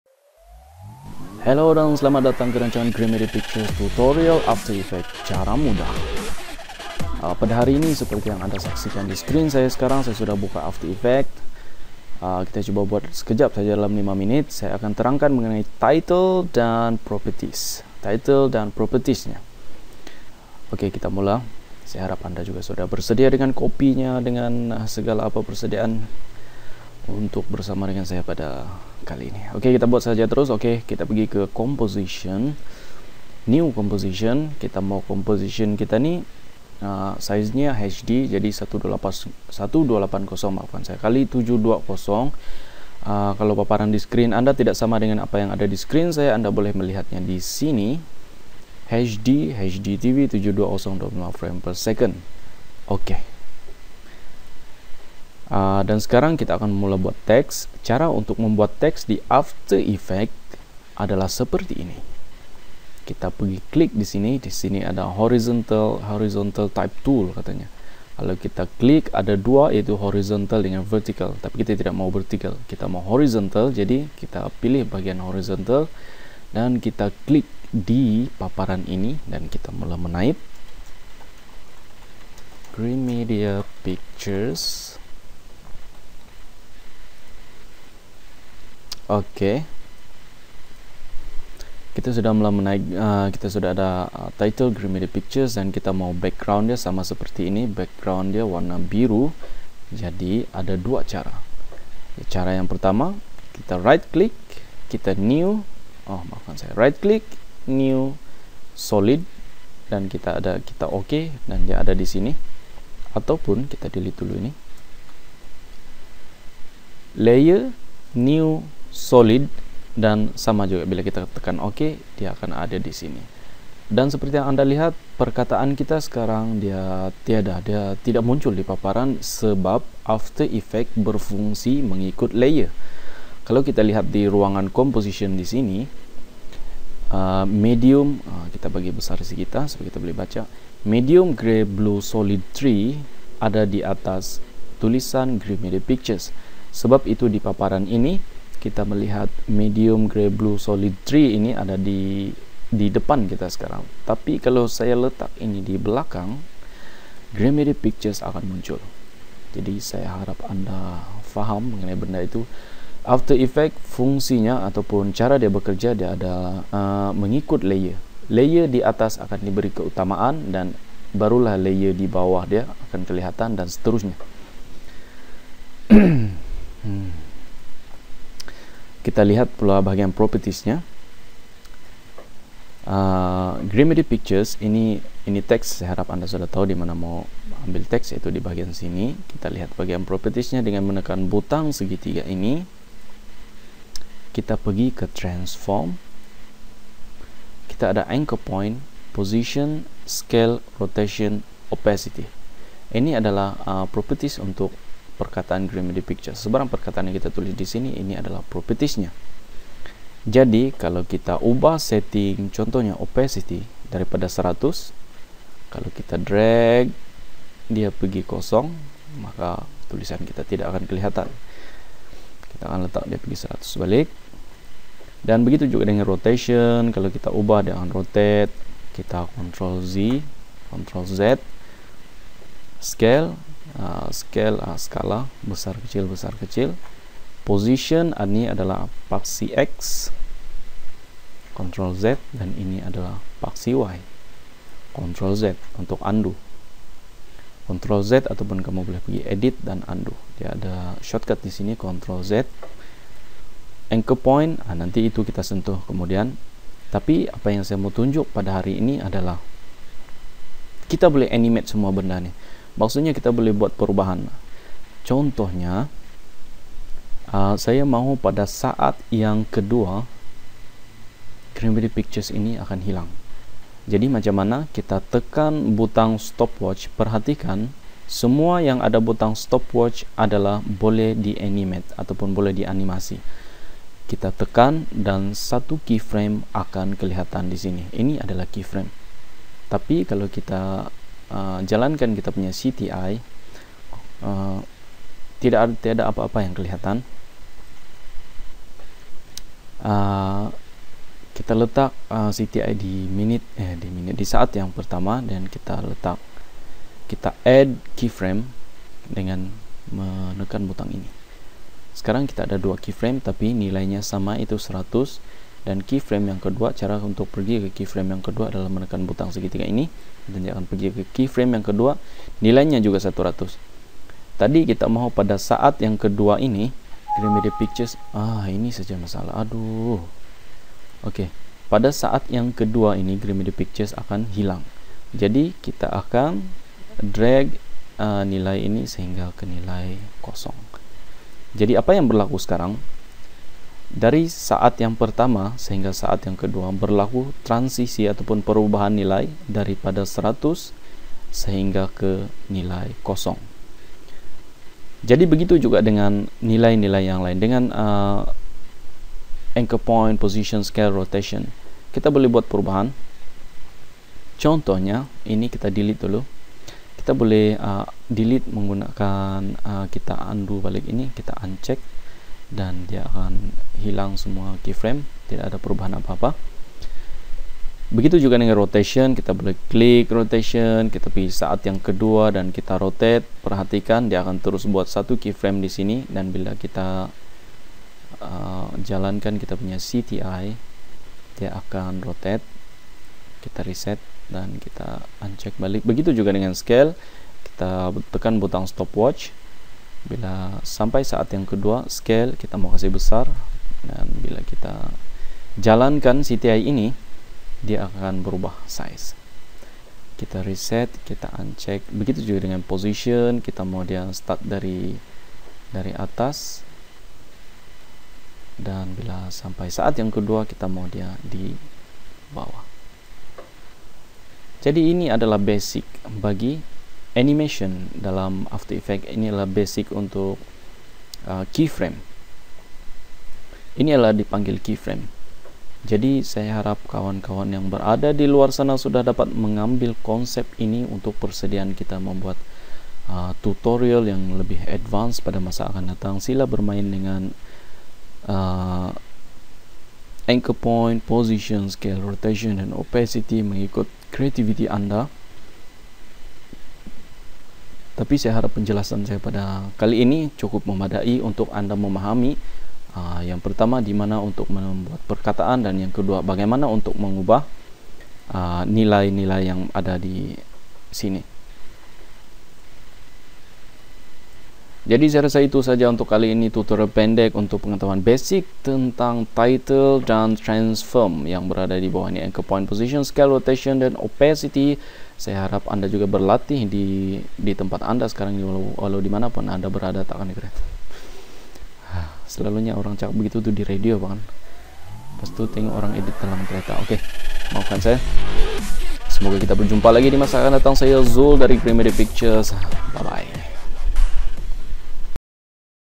Gremedia Pictures Ep 3: Titles dan Properties
After effect cara mudah dengan Gremedia Pictures: Fokus kali ini adalah cara membuat titles dan cara ubah properties/setting tulisan.